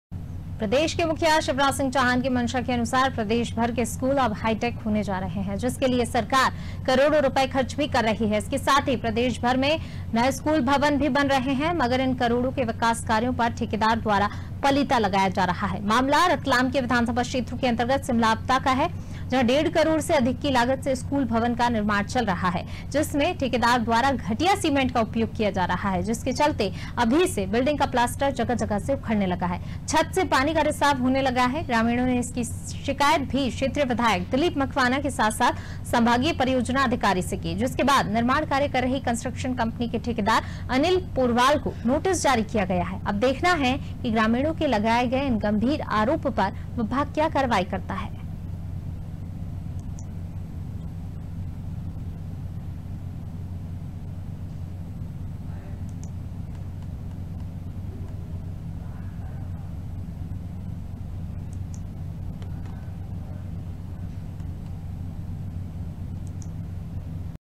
रतलाम: स्कूल भवन का घटिया निर्माण कार्य |Aaj ki Taza Khabar | Latest Hindi news update | Breaking
प्रदेश के मुखिया शिवराज सिंह चौहान की मंशा अनुसार प्रदेश भर के स्कूल अब हाईटेक होने जा रहे हैं जिसके लिए सरकार करोड़ों रुपए खर्च भी कर रही है... साथ ही प्रदेश भर में नए स्कूल भवन भी बन रहे हैं...मगर इन करोड़ों के विकास कार्यों पर ठेकेदार द्वारा पलीता लगाया जा रहा है...मामला रतलाम के विधानसभा क्षेत्र के अंतर्गत सिमलावदा का है...जहां डेढ़ करोड़ से अधिक की लागत से स्कूल भवन का निर्माण चल रहा है...जिसमें ठेकेदार द्वारा घटिया सीमेंट का उपयोग किया जा रहा है...जिसके चलते अभी से बिल्डिंग का प्लास्टर जगह-जगह से उखडने लगा है. छत से पानी रिसाव होने लगा है... ग्रामीणों ने इसकी शिकायत भी क्षेत्रीय विधायक दिलीप मकवाना के साथ साथ संभागीय परियोजना अधिकारी से भी की है...जिसके बाद निर्माण कार्य कर रही कंट्रक्शन कंपनी के ठेकेदार अनिल पोरवाल को नोटिस जारी किया गया है...अब देखना है ग्रामीणों के लगायें गयें इन गम्भीर आरोप पर विभाग क्या कार्यवाही करता है...